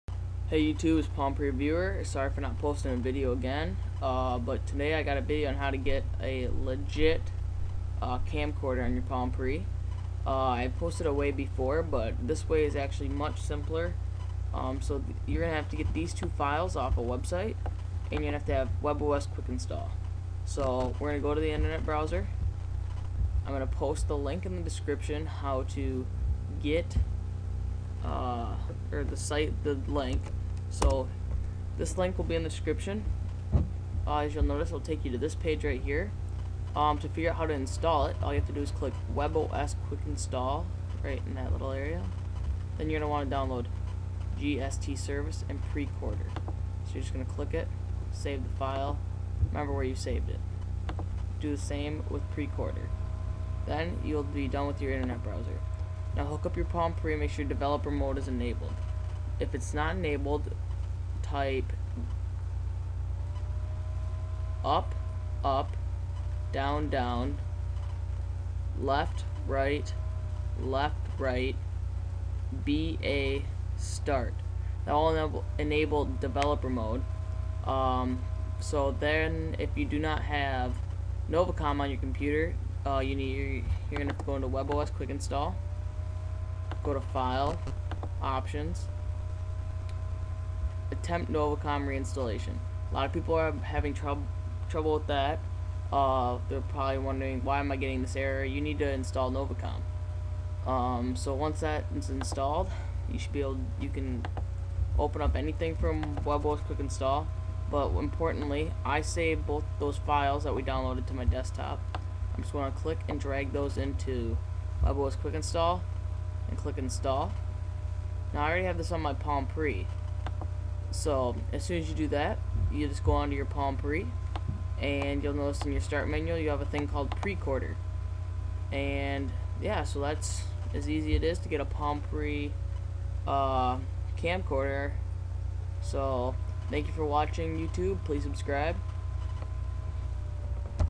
How To Get Precorder On Palm Pre
This video is on how to get Precorder on your palm pre. Precorder is a camcorder.